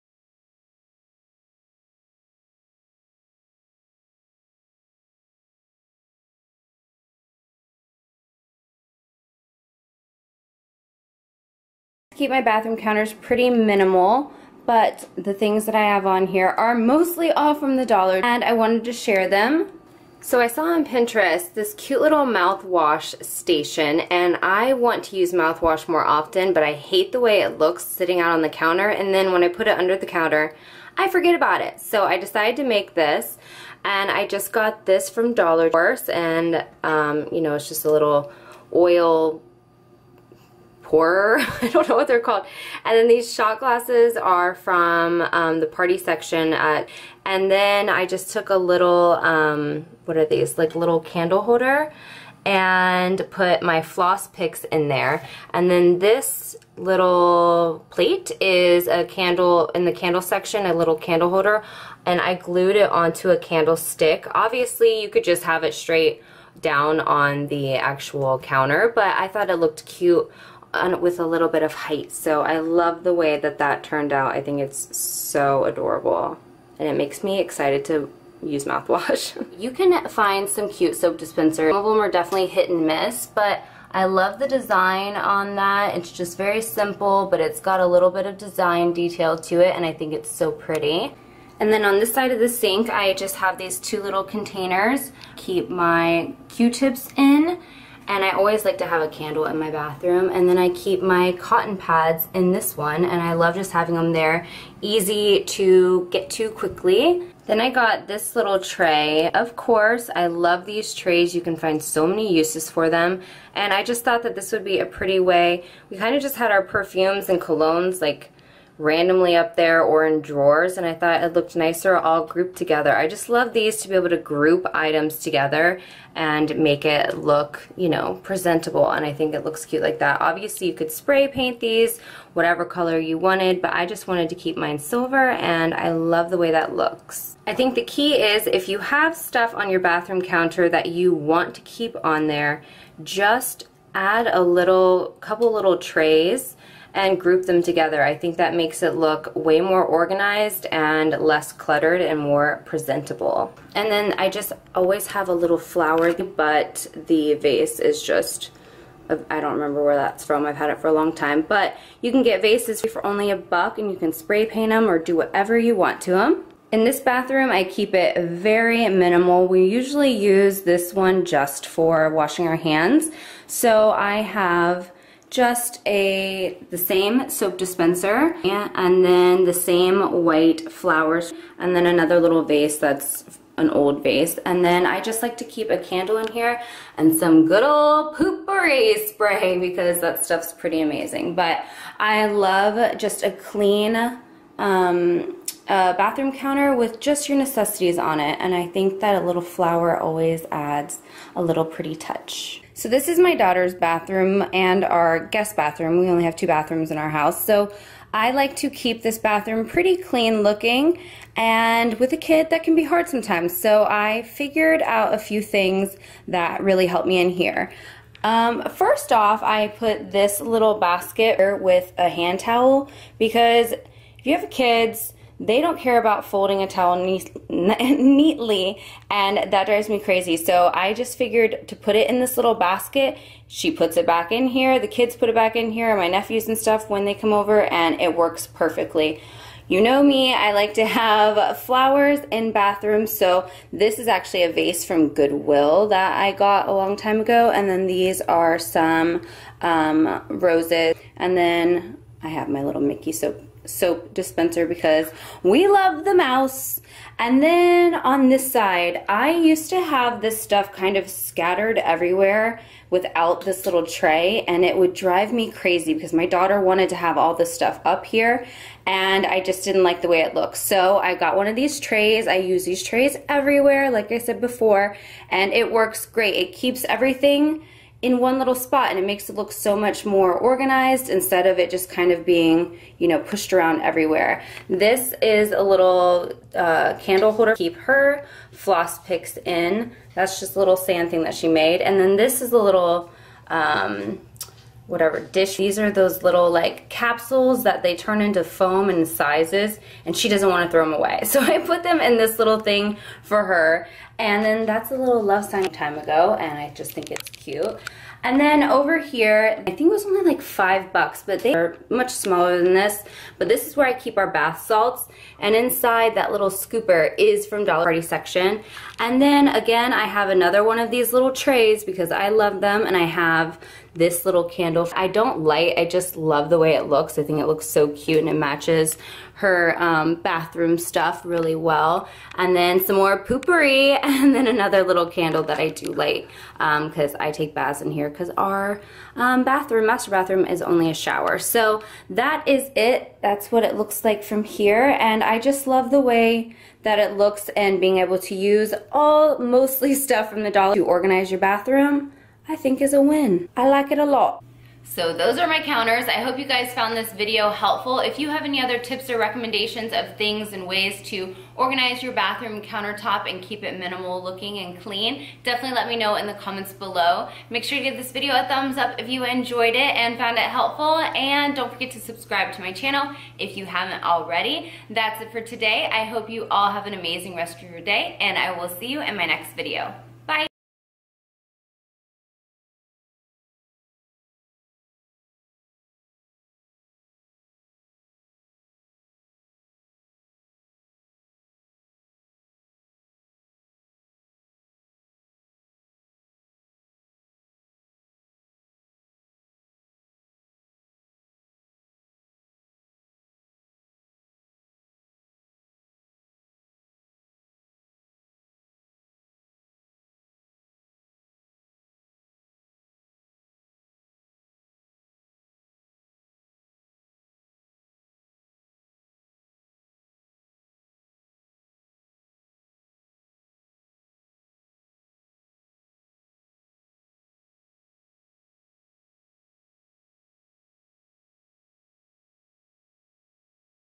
Dollar store home decor ideas|Discount store decorating|UF|Unusual Finds
dollar store diy room decor
Easy DIY DOLLAR STORE HOME DECOR! SENSATIONAL FINDS when it comes to Unusual Finds
there is more than enough to find adn play with.
Easy DIY DOLLAR STORE bathroom decor is on this video
DIY $1 ROOM DECOR!|
make yourself a bathroom mouth wash center with dollar store items plus decorate your bathroom with other dollar store things.
Silver colored trays look like a millionaire dollar and are an easy home decor secret to clean up your collections and make them look good. Spray paint them or leave them chromed silver. Square circle of oblong all will look good.
1-Take a glass pillar candle holder and hot glue to a vase or decorative glass and turn it upside down to become your base.
2-find yourself a decorative glass with ornate outside and place your dollar toothpicks in them.
3-Stack plastic through away or paper shot glasses upside down ready to use
4-Oil bottle to fill up your mouthwash.
This makes your ugly mouth wash look great. Gargle away.

Extras-
Pick up some candles. Soft and sexy for soaking in a hot bath.
basket for used to towels kids uses because children tend to not hang up.
Extra trays for perfumes or other items.
Containers with lids for q-tips adn cotton pads storage.
Vases
Long stem flowers
Discount stores with have storage containers for salt etc for soaking in the bath